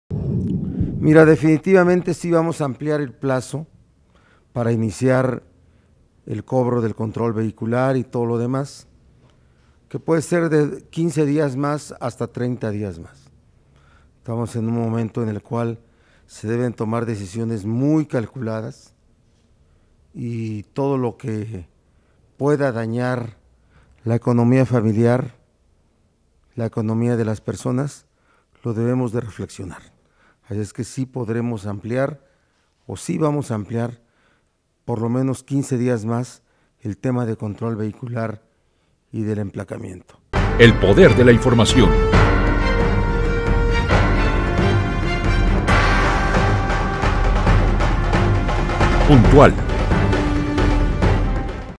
Otra prórroga para el pago de control vehicular en Puebla
Con el propósito de no afectar a la economía de las familias poblanas durante la emergencia sanitaria, el gobernador Miguel Barbosa Huerta anunció que se ampliará de 15 a 30 días el plazo para pagar el control vehicular y el reemplacamiento. En conferencia de prensa el gobernador indicó que la decisión de ampliar el plazo de cobro se realizó por las condiciones económicas que se viven actualmente a raíz de la pandemia de Coronavirus.